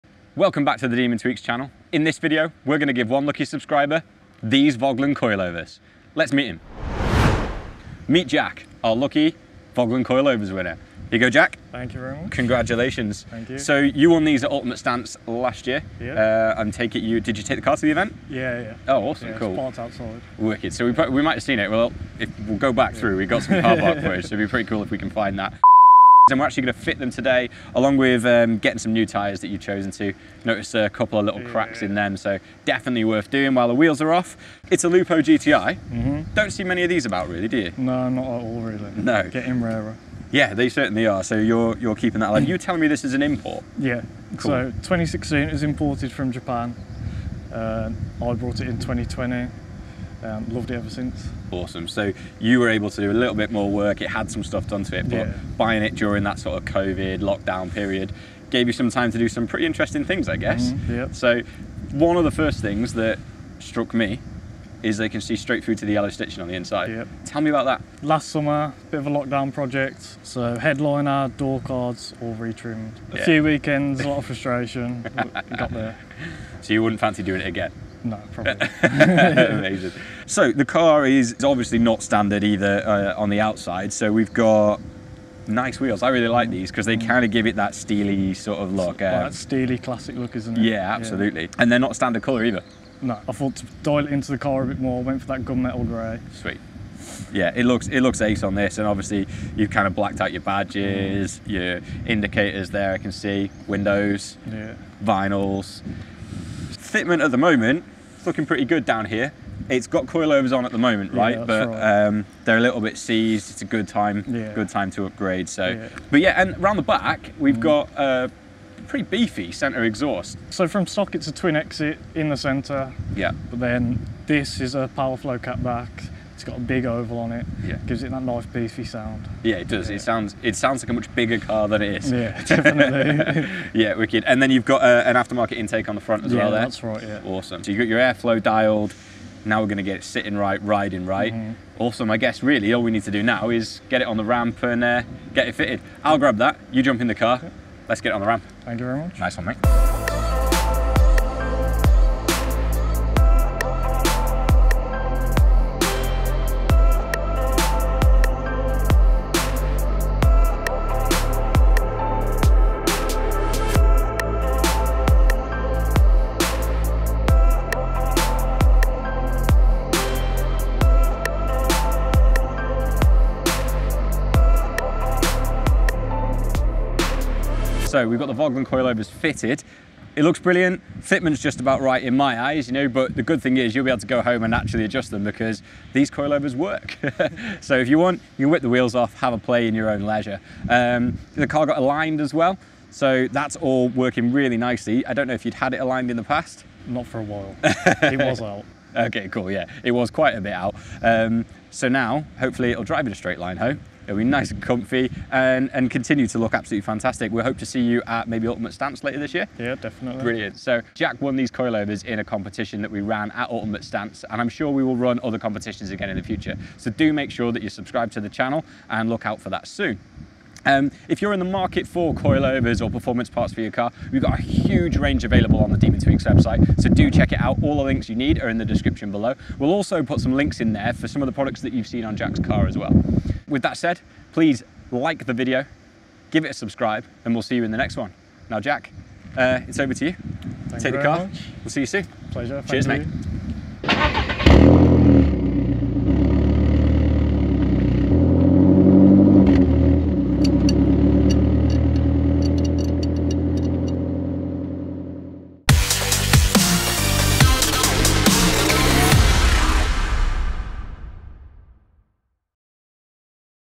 Giving Away Vogtland Coilovers to a Subscriber! | VW Lupo GTI | Demon Tweeks
In this video our competition winner Jack gets his new Vogtland coilovers fitted to his modified VW Lupo GTI. We take a look around his car and discuss the modifications from intake & exhaust to wheels & tyres, and of course we discuss the suspension too!

Chapters:
00:00 Winning Vogtland Coilovers
00:26 Modified VW Lupo GTI
01:01 Custom Interior and Door Cards
01:25 ATS Cup Wheels
01:57 Custom Powerflow Exhaust
02:14 BMC Air Intake
02:29 Fitting Vogtland Coilovers
02:58 Finishing Up
03:31 How You Can Win